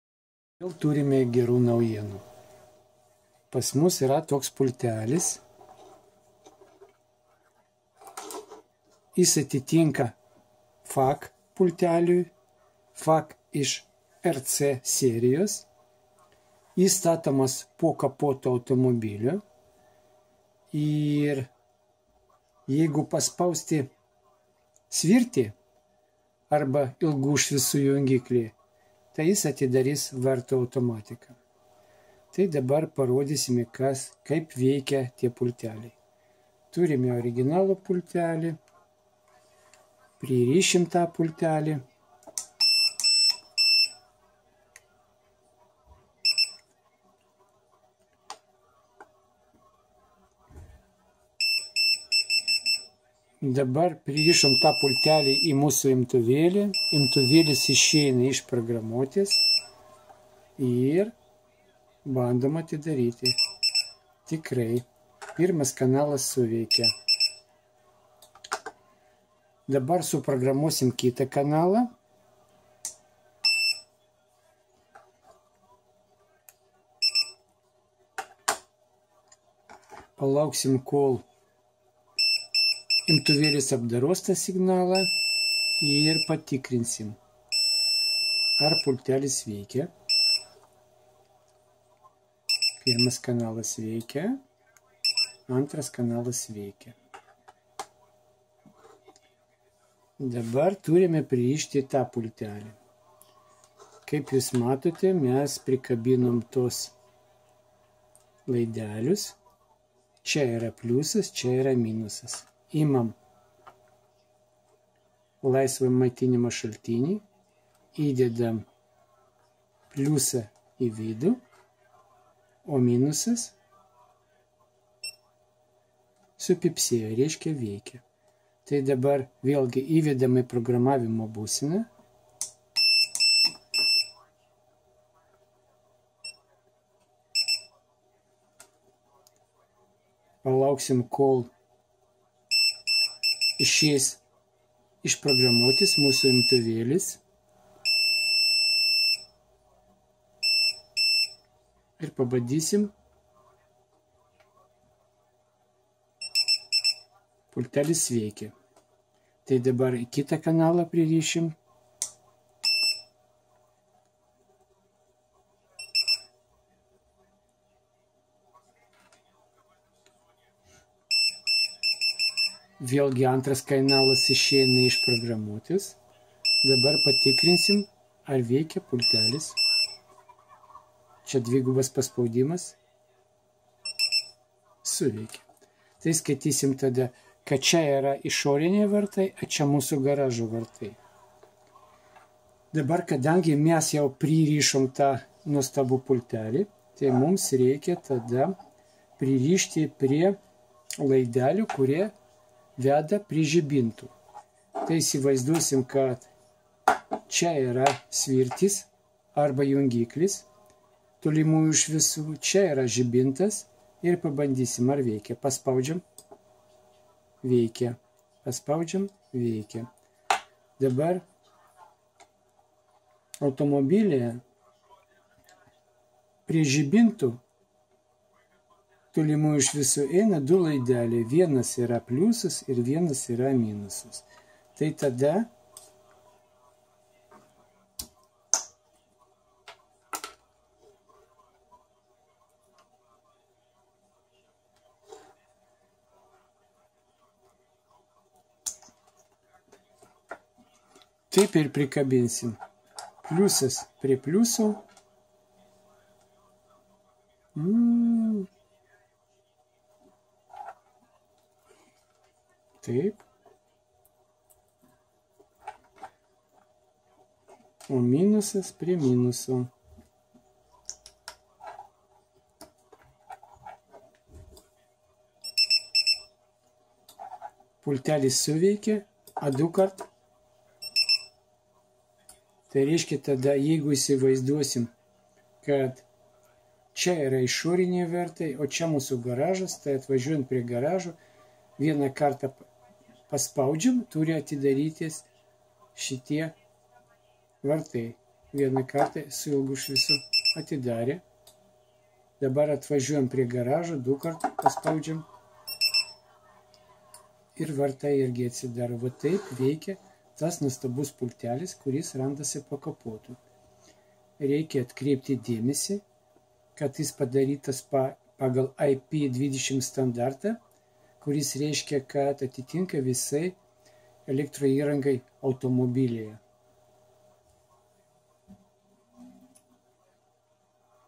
Faac RC automatika atsiverianti nuo tolimosios automobilio šviesos
FAAC  nuotolinio valdymo pultas, skirtas montuoti automobilyje. Pavyzdžiui, prijungę jį prie tolimųjų šviesų jungiklio, galėsime atidaryti vartus, trumpam mirksėdami tolimųjų šviesų žibintus. FAAC nuotolinio valdymo pultas yra keturių kanalų nuotolinio valdymo pultas, o tai reiškia, kad mirksint žibintams vieną, du, tris ar keturis kartus kodas bus išsiųstas atitinkamai į pirmąjį, antrąjį, trečiąjį ar ketvirtąjį nuotolinio valdymo pulto mygtuką.
       Nuotolinio valdymo pultą galima prijungti taip, kad jo nebūtų galima įjungti, kai automobilis neįjungtas. Nuotolinio valdymo pultas yra E20 patvirtintas montuoti transporto priemonėje.
        Taip pat yra kitų žinomų gamintojų nuotolinio valdymo pultai